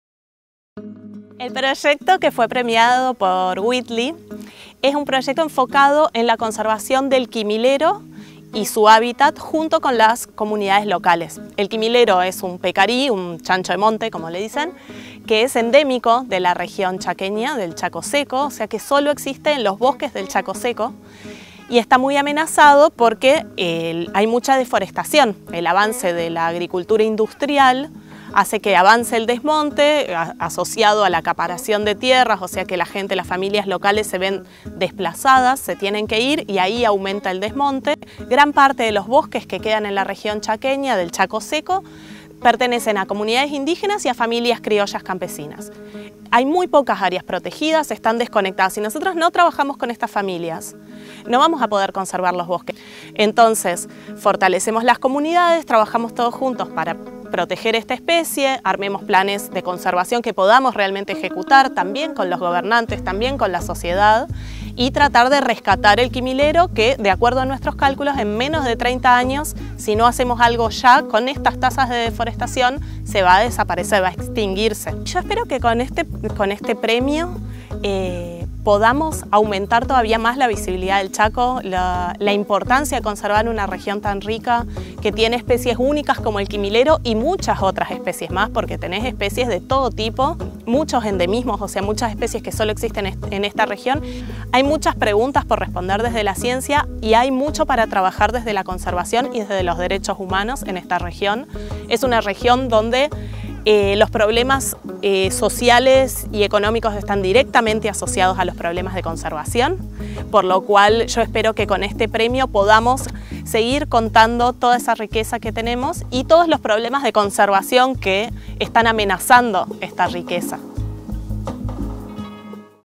Una investigadora del CONICET ganó el “Oscar Verde” por su proyecto de conservación
Micaela Camino, investigadora del CONICET en el Centro de Ecología Aplicada del Litoral (CECOAL, CONICET - UNNE) fue una de las ganadoras de los premios Whitley que se entregaron hoy en Londres. Este galardón es conocido como el “Oscar Verde” y distingue a proyectos de conservación orientados a países ricos en biodiversidad.

La propuesta premiada tiene el objetivo de aumentar las capacidades de las comunidades locales para conservar el quimilero y el Chaco argentino.  El pecarí quimilero (Catagonus wagneri) es una especie endémica de la región del Chaco seco que está amenazada y que, de acuerdo a estimaciones de la investigadora, podría desaparecer en menos de 30 años.